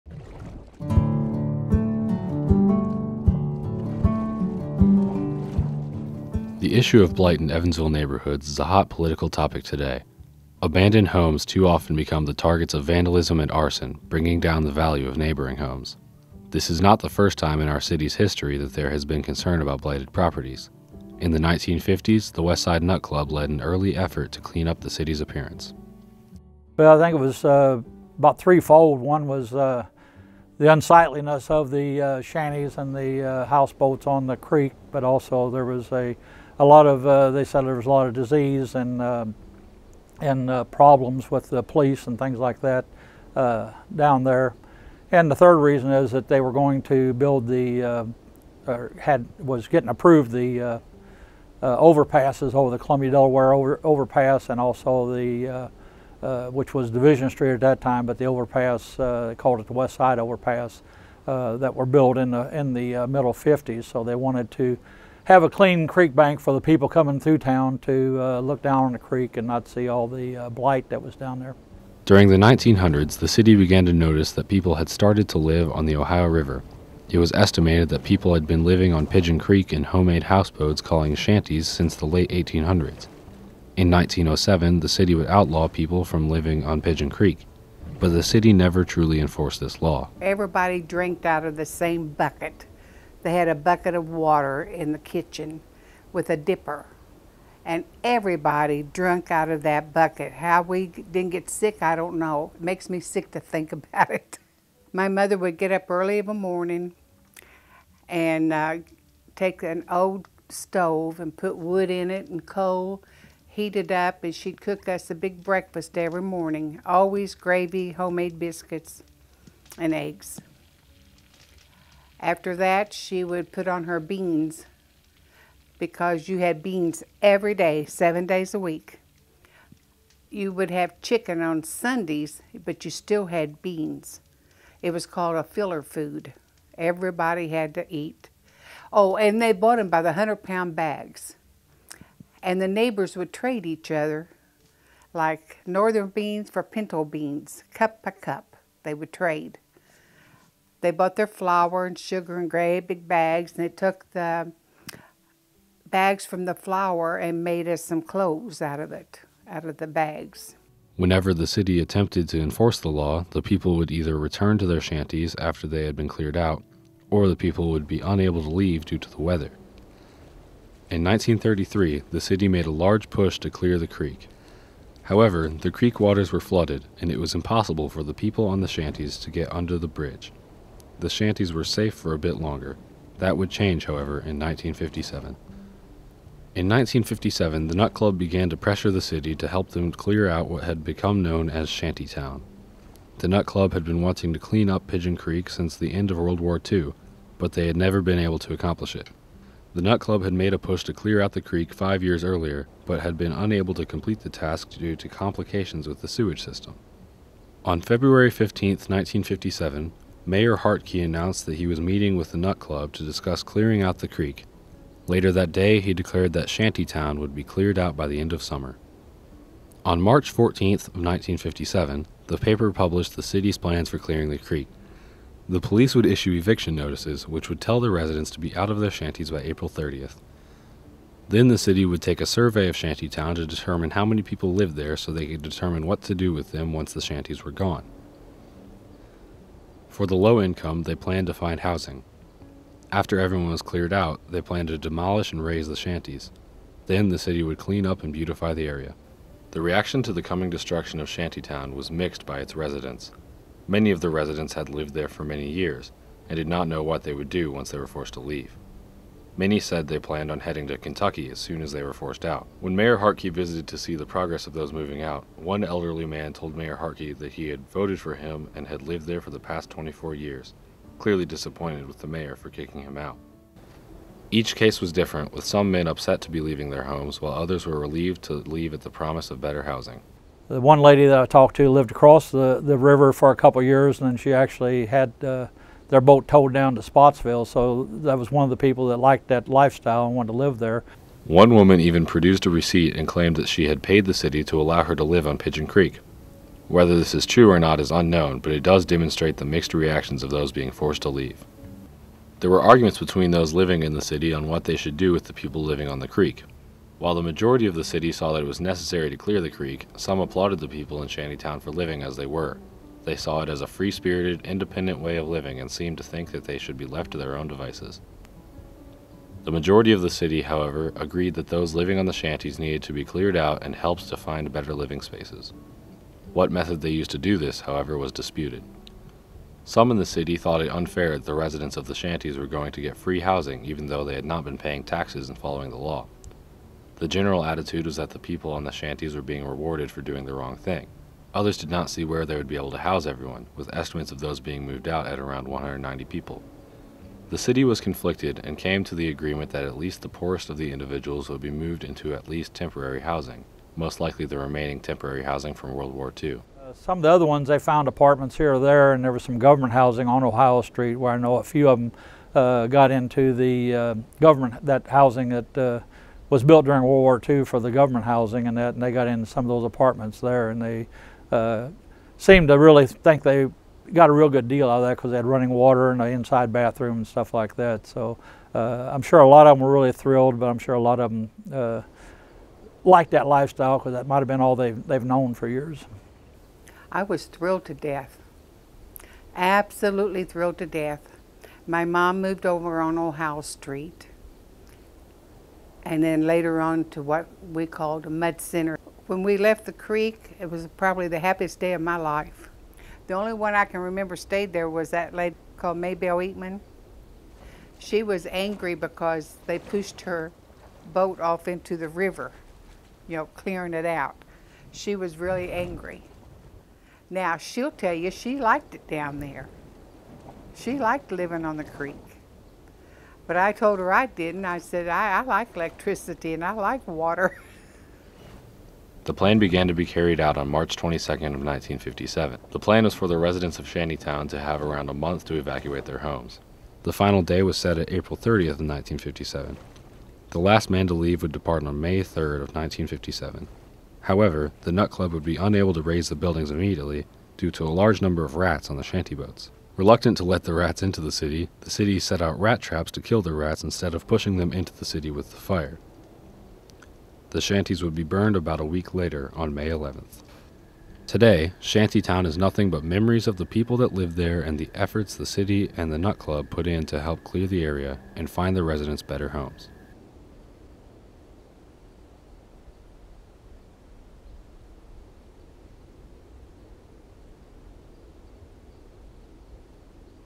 Pigeon Creek Shanty Town: The Story of a Lost Community
This is a documentary produced by the students of the Feel the History class at F.J. Reitz High School in Evansville Indiana.  This video focused on the shanty boat community of Pigeon Creek and the cleanup effort of city officials and the West Side Nut Club in 1957.  Students produced all parts of the finished product.